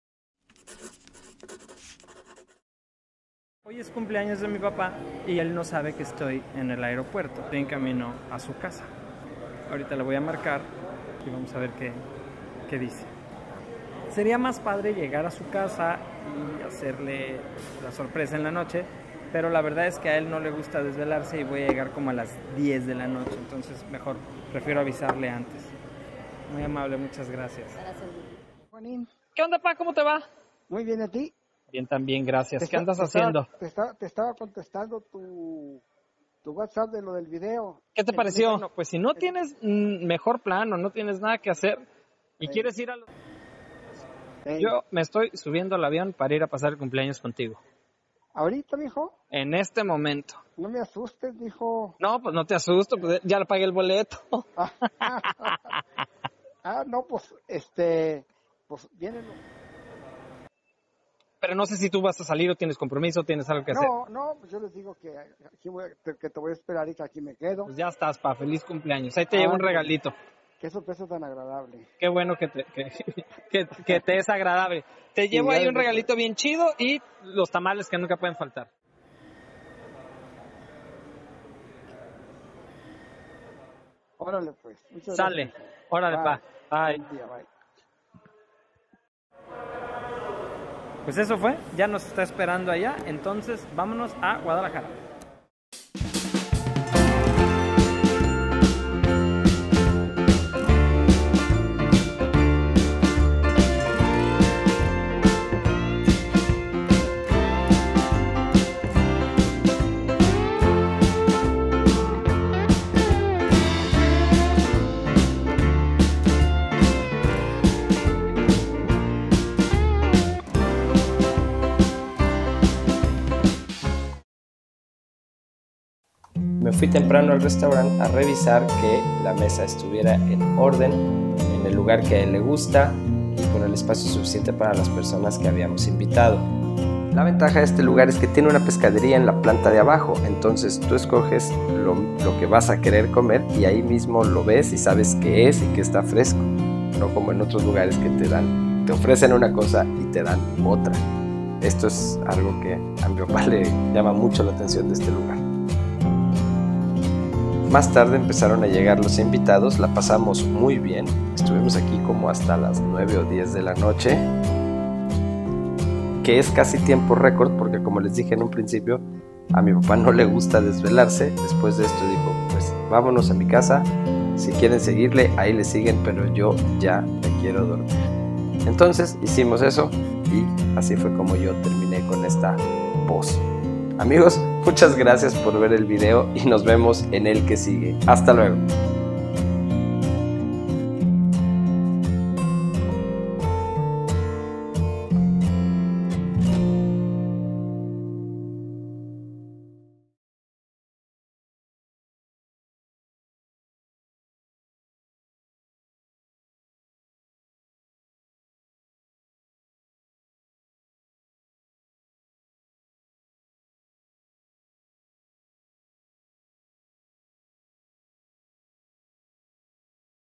El Cumpleaños de mi papa.
Fuimos a comer con mi papa por su cumpleaños.